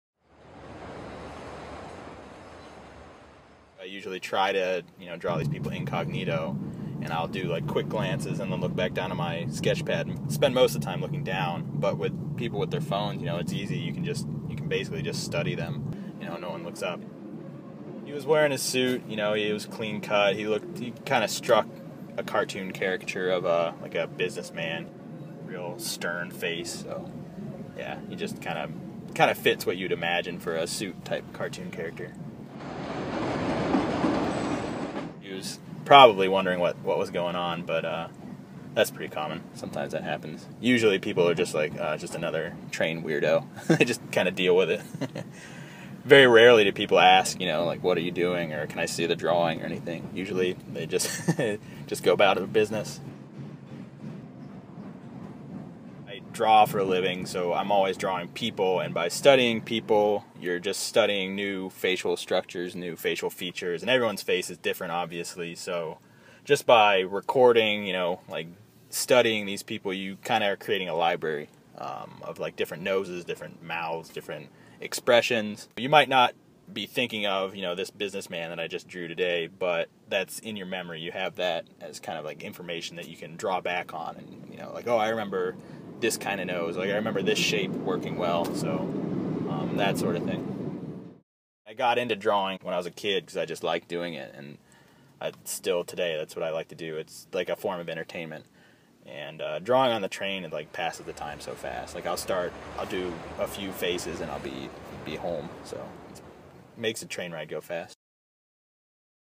Artist Eric Meister
Eric Meister likes sketching people while riding on the train.